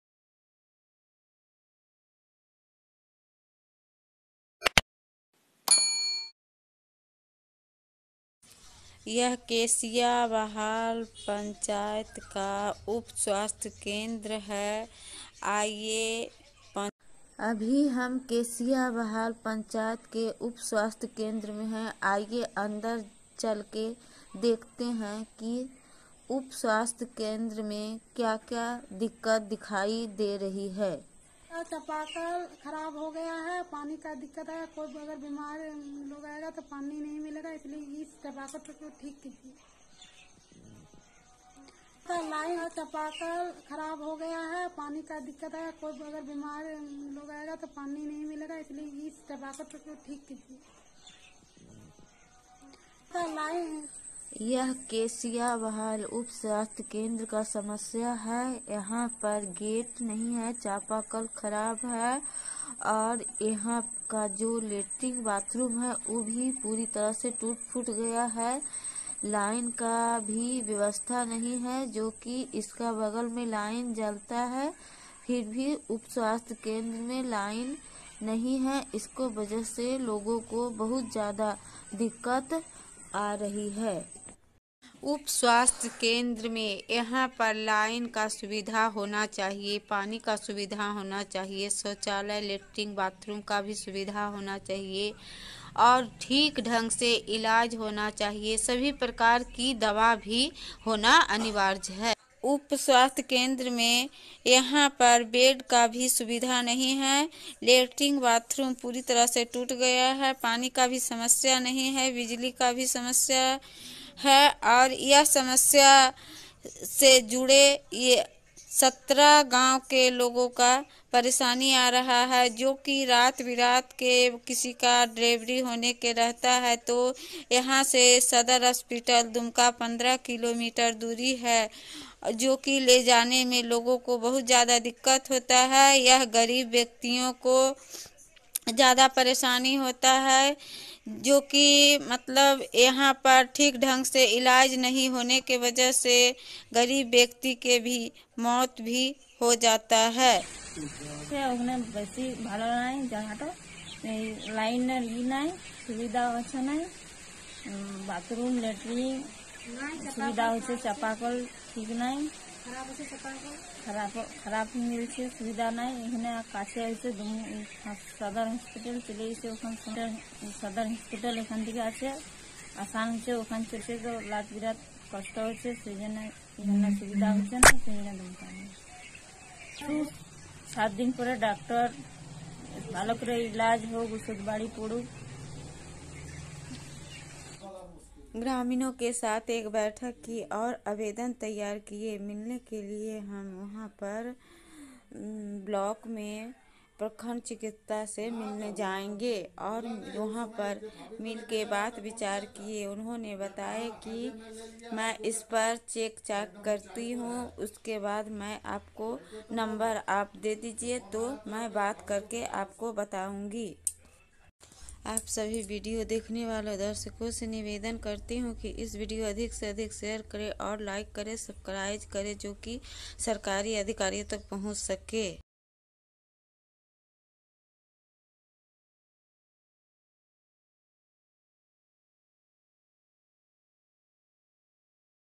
दुमका उप स्वास्थ्य केंद्र में सुविधाओं की कमी का दंश झेल रहे मरीज़
ग्राम केसिया बहाल पंचायत केसिया बहाल प्रखंड दुमका जिला दुमका के  उप स्वास्थ्य केंद्र में लगभग 10-15 साल से बिजली की सुविधा नहीं है। वहीं बाथरूम और पानी का भी सुविधा नहीं है। अस्पताल में बेड की कमी के साथ उचित इलाज भी नहीं होता है।

This story is by Sanchita Pathak, a Community, Content Creator from Dumka, Jharkhand.